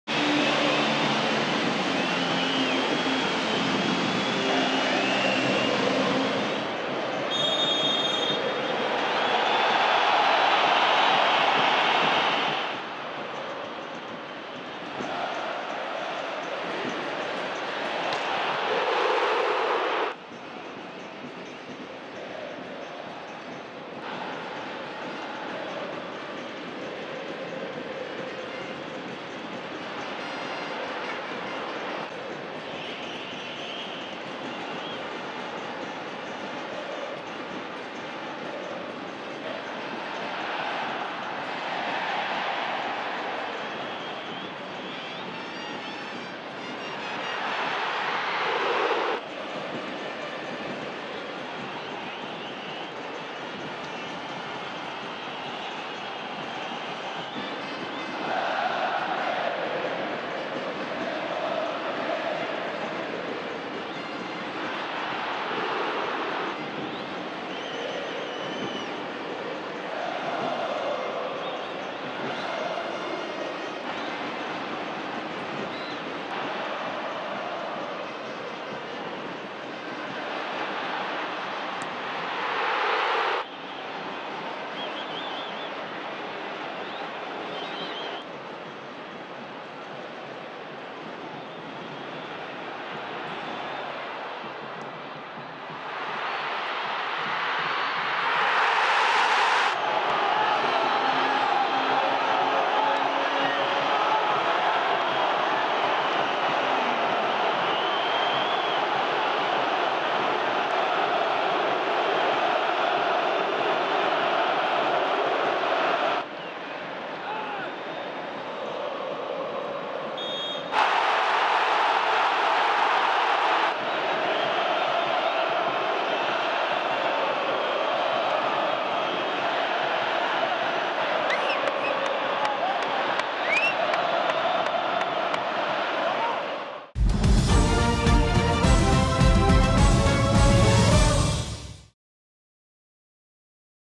INTERNACIONAL X METROPOLITANOS | MELHORES MOMENTOS | CONMEBOL LIBERTADORES 2023
🏆🔴⚪ Festa no Gigante da Beira-Rio! Com gol nos acréscimos, o Internacional venceu o Metropolitanos por 1-0 e somou sua primeira vitória no Grupo B da CONMEBOL Libertadores.